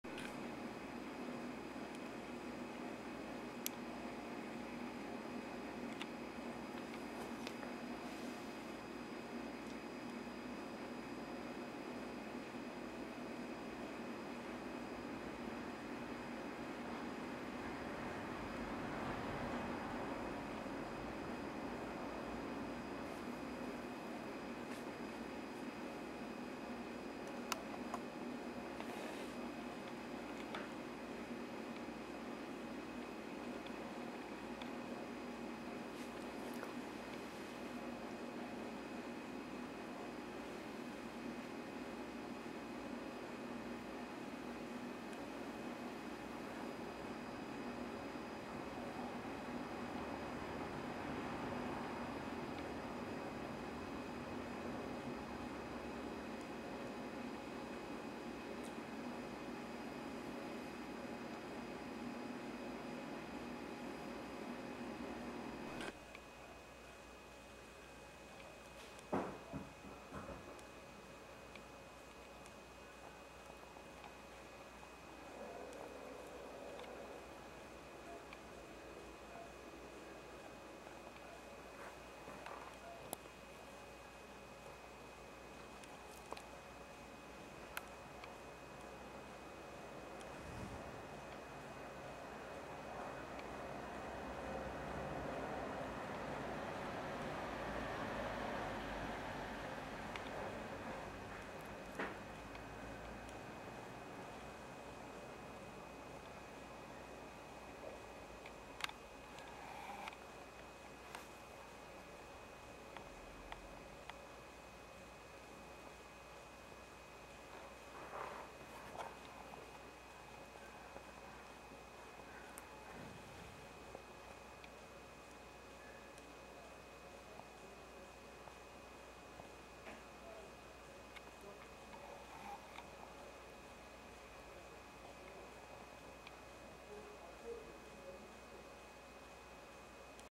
Eresia lansdorfi Butterfly - Lansdorf's Crescent (Melitaeini) Florianópolis
Eresia lansdorfi - Lansdorf's Crescent - False Erato (Nymphalidae - Nymphalinae - Melitaeini - Phyciodina) butterfly species, Borboleta Eresia, Natureza, Nature, Biodiversidade, Florianópolis, Santa Catarina, Brasil

Eresia is a genus of butterflies from Mexico to South America in the family Nymphalidae.

alike to Heliconius species